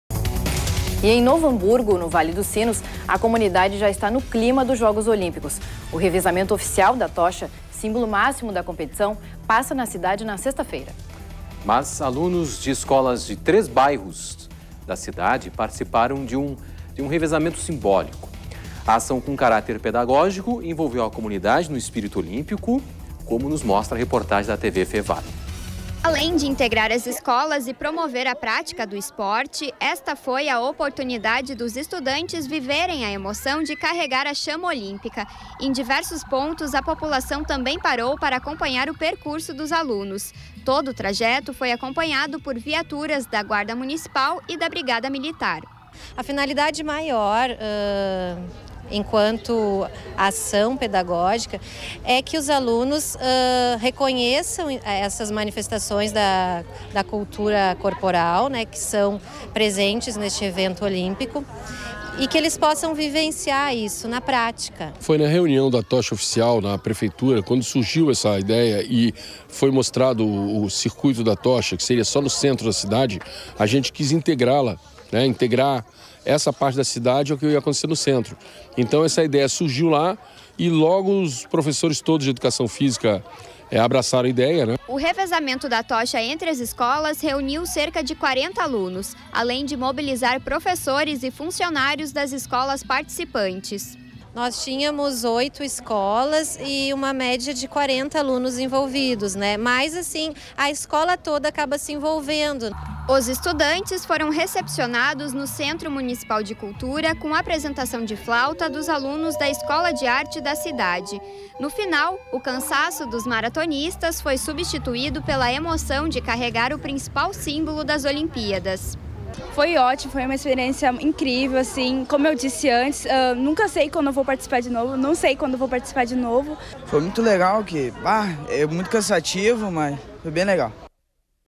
Novo Hamburgo se prepara para passagem da Tocha Olímpica
Na contagem regressiva para os jogos do Rio de Janeiro, a expectativa aumenta e a Tocha Olímpica segue pelo interior do Estado. Confira.